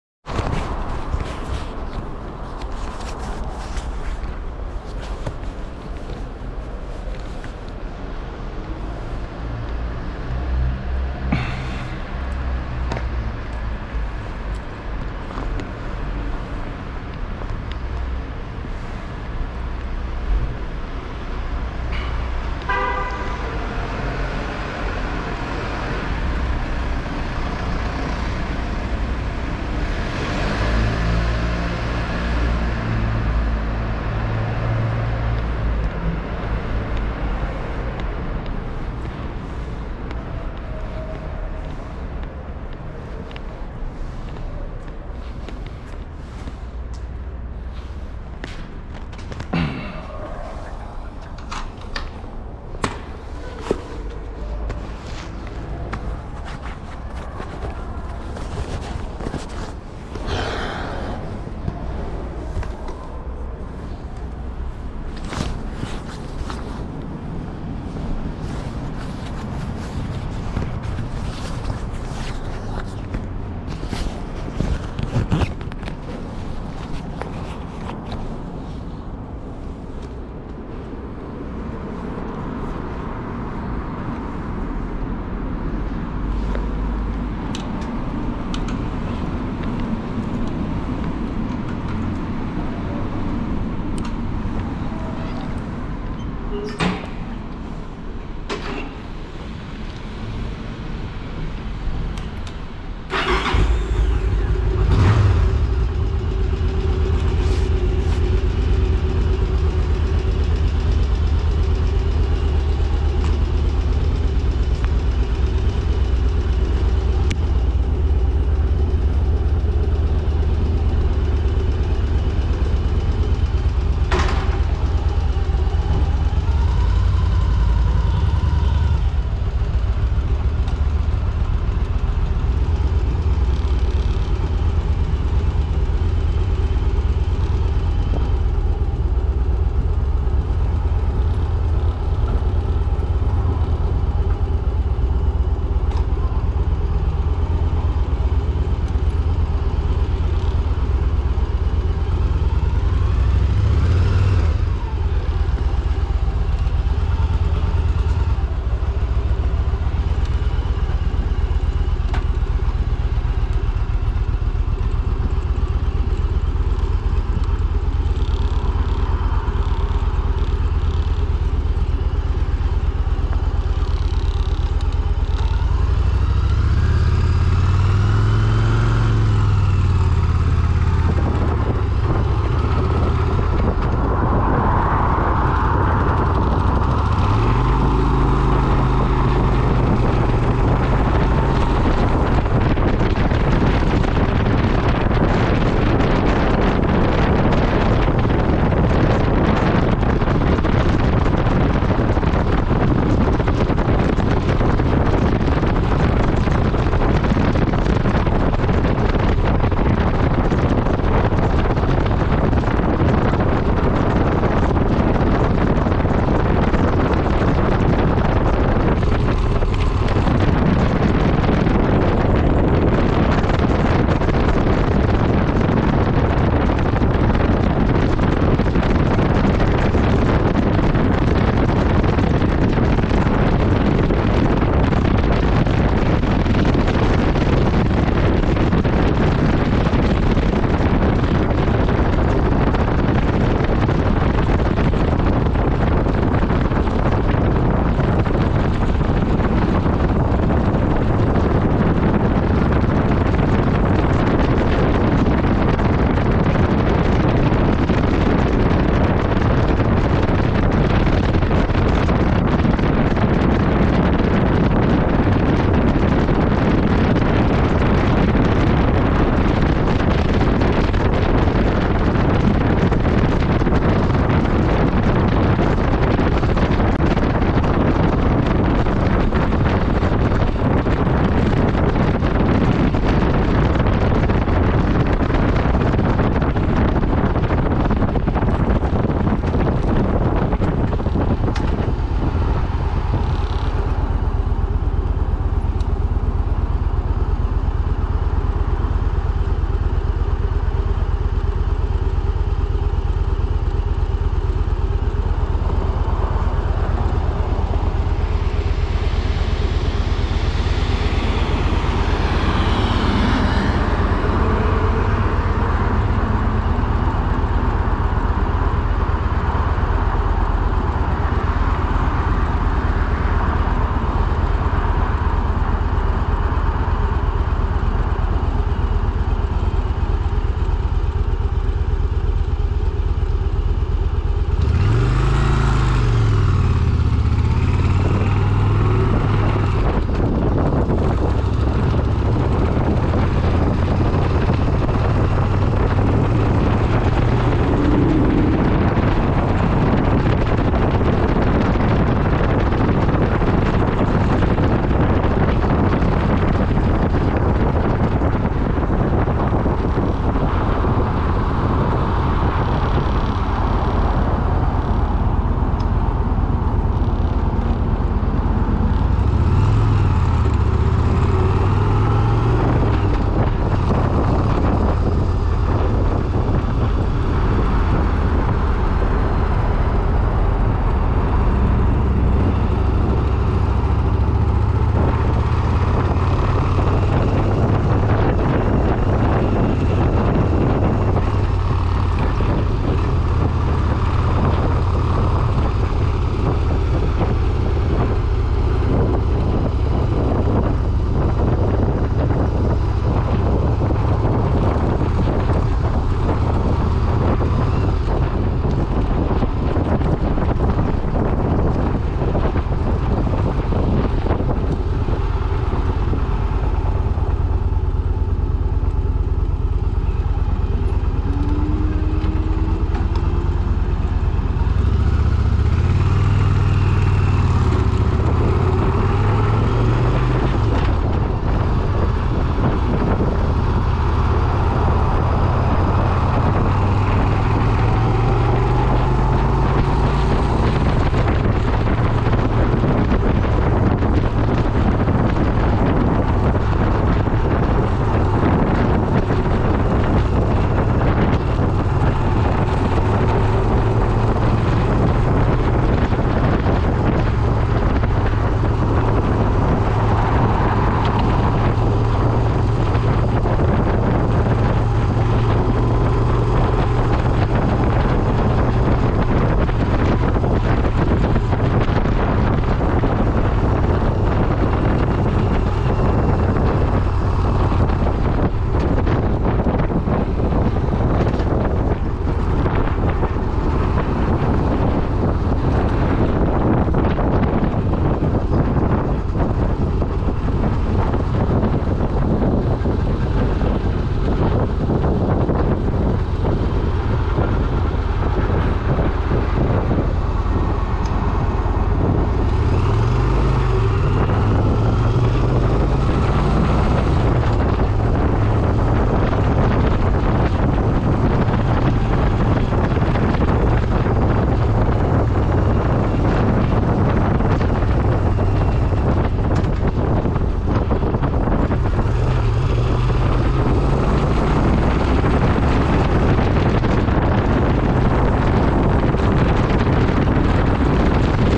Drive Test Westcreek to Kaiser :-) Yamaha V Star 1100cc Silverado
Yamaha V Star 1100cc Silverado model 2004 Cruiser Big Bike Test Ride